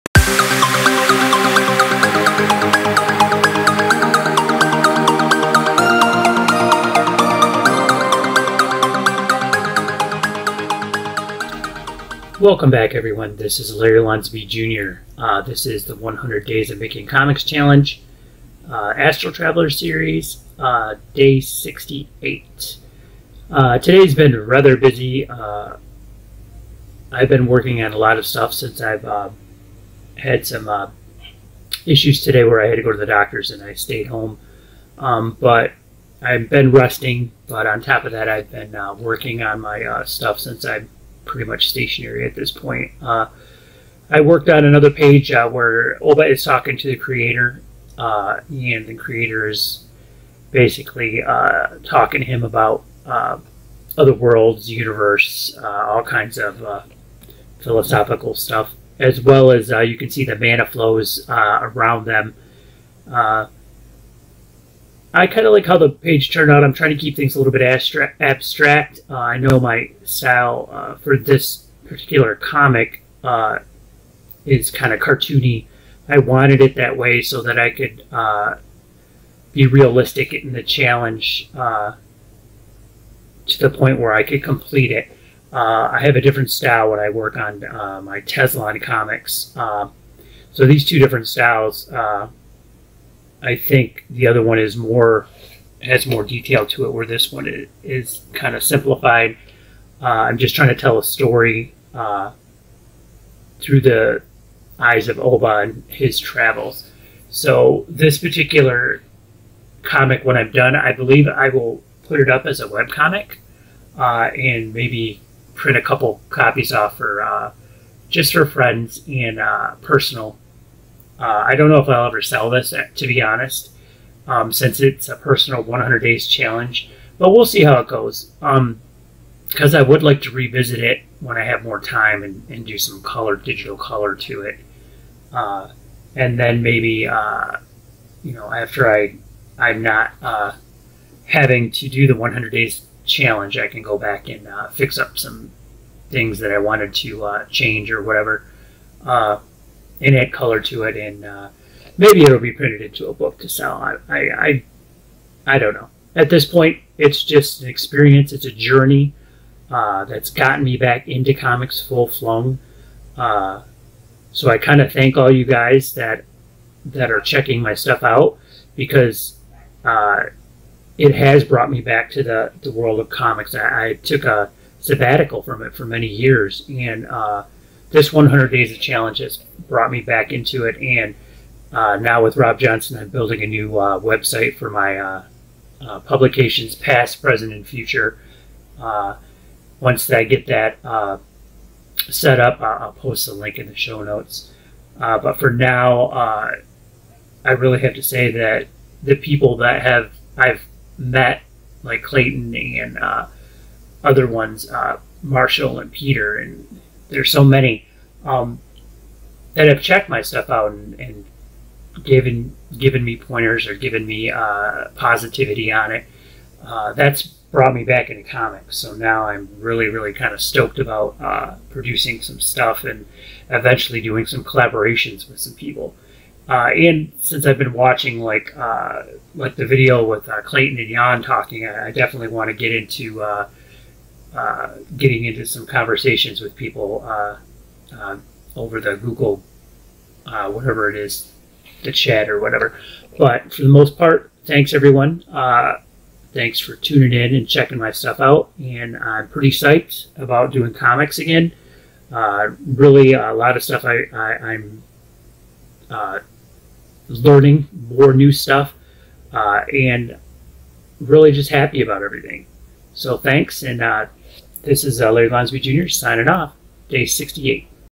100 Days of Making comics Season 1 Day 68 6/21/2019
100 Days of Making Comics-Astral Traveller Day one-This is my first attempt at the challenge by Kevin Cross (100 Days of making comics)

The journey of Oba unfolds via his travels through the inner and outer worlds of reality-the Astral planes.